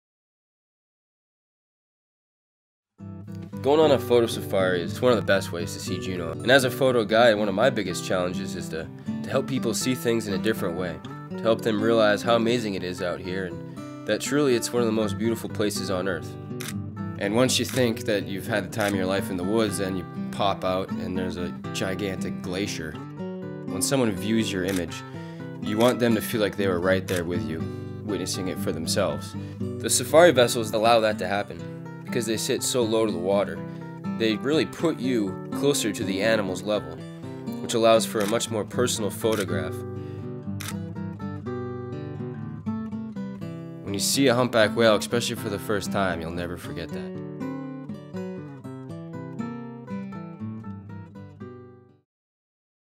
Whale Watching and Mendenhall Glacier Photo Safari
Create memories to last a life time and capture them all on film!  From the most advanced photographers to those just getting started, our Whale Watching and Mendenhall Glacier Photo Safari is a great way to see the wild and capture the wonder of Southeast Alaska. As you travel with a professional naturalist photographer within a small group of 14 or less immerse yourself into the top destinations of Juneau as you visit the trails at Mendenhall Glacier and board a custom designed safari vessel to explore and capture on film the abundant wildlife and landscapes that make Juneau a top destination in Southeast.